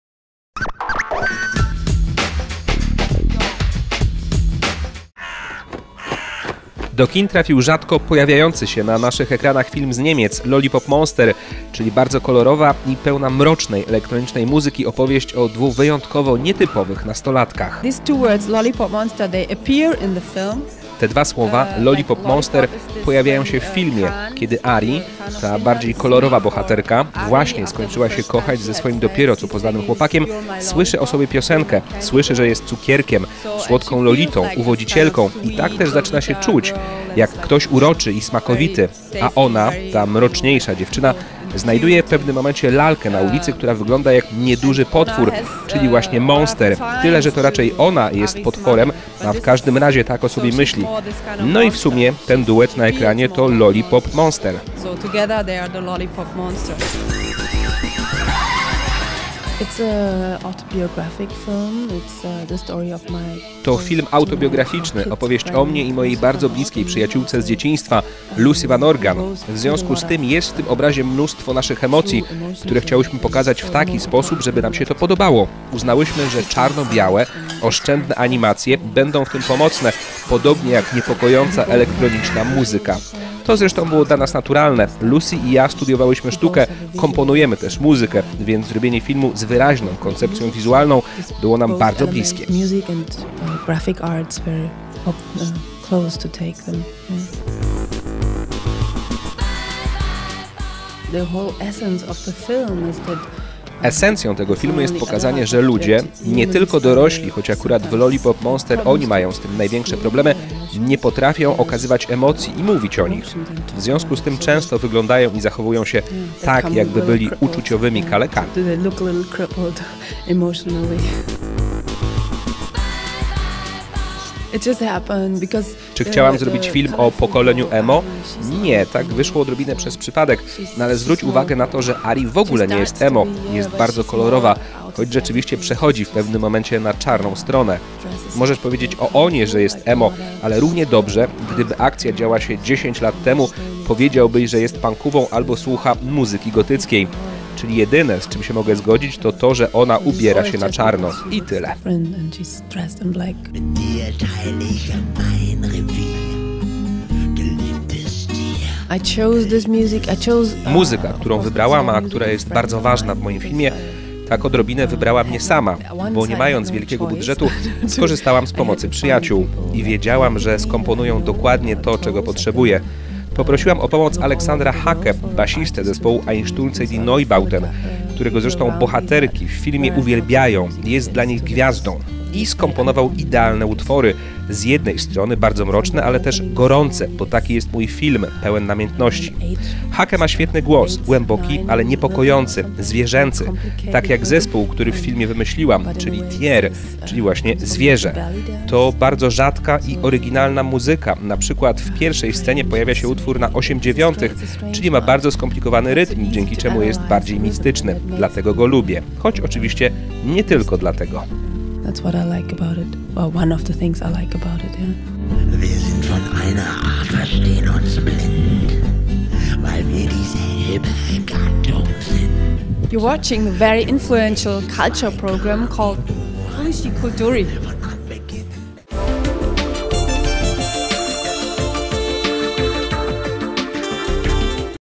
Ziska Riemann - reżyserka filmu "Lollipop Monster" gościem "KulisKultury"!
„Lollipop Monster" ciepło przyjęto na festiwalach w Berlinie i Karlowych Warach. Teraz ta momentami mroczna, a momentami kiczowato kolorowa opowieść o dwu dziwnych nastolatkach z dwu bardzo dziwnych rodzin dotarła do Polski. Reżyserką jest Ziska Riemann, która do spółki ze współscenarzystką Luci Van Org, opowiedziała nieco autobiograficzną historię z czasów dzieciństwa tworząc z niej współczesną bajkę w stylu pop. To rzadki na naszych ekranach przykład współczesnego i eksperymentującego kina z Niemiec dlatego, choć na pierwszy rzut oka wydaje się to propozycja dla młodszej widowni, warto spędzić przed ekranem 90 minut i obejrzeć „Lollipop Monster".